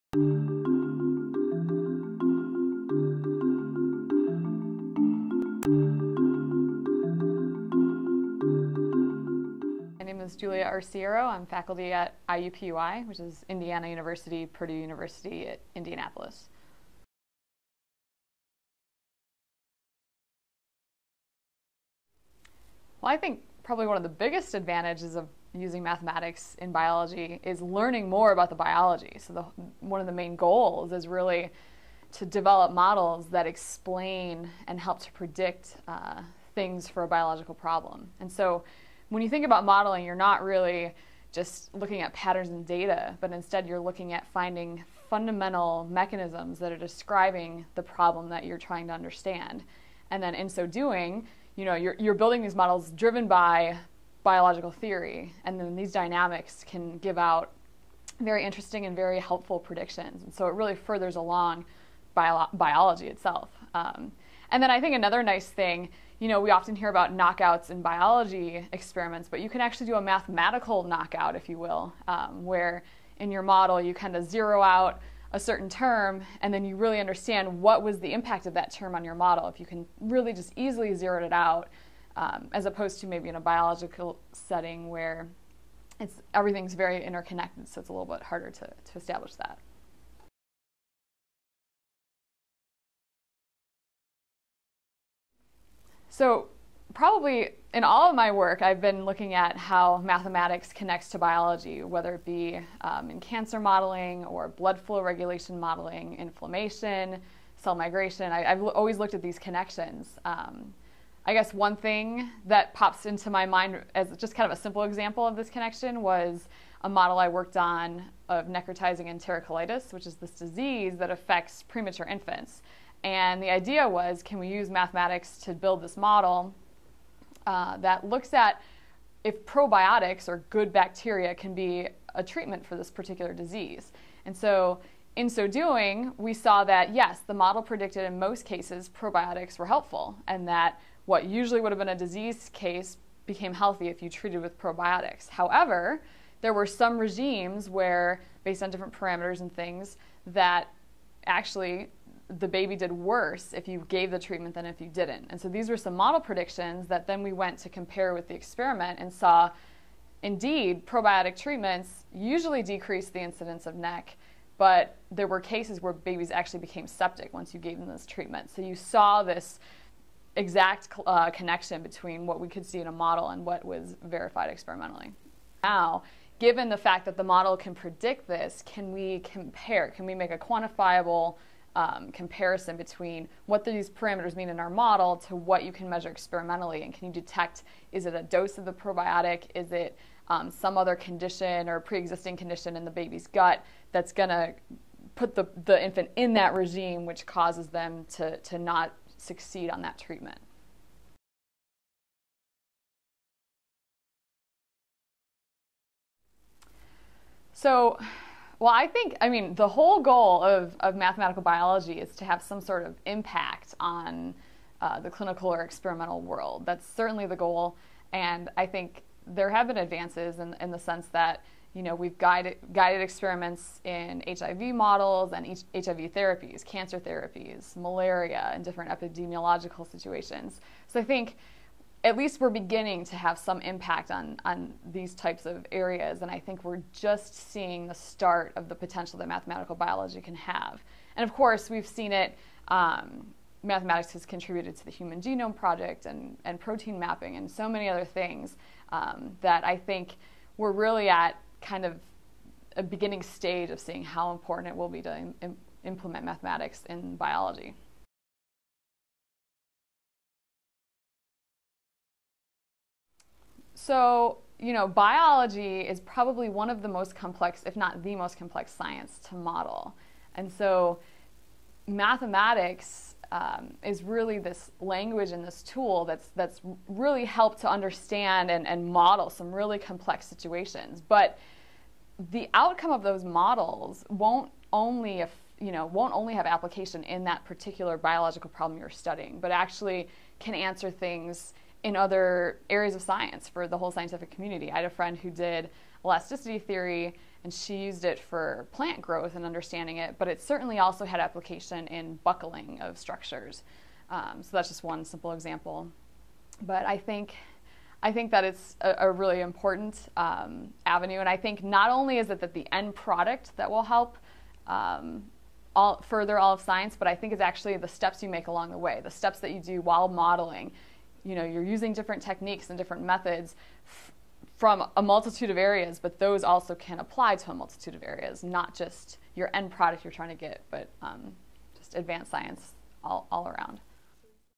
Learning Biology With Mathematics, Dr. Julia Arciero
In an interview at the National Institute for Mathematical and Biological Synthesis, Dr. Julia Arciero, an assistant professor of mathematics at Indiana University-Purdue University Indianapolis, explains how mathematical biology has applications to all other areas of science.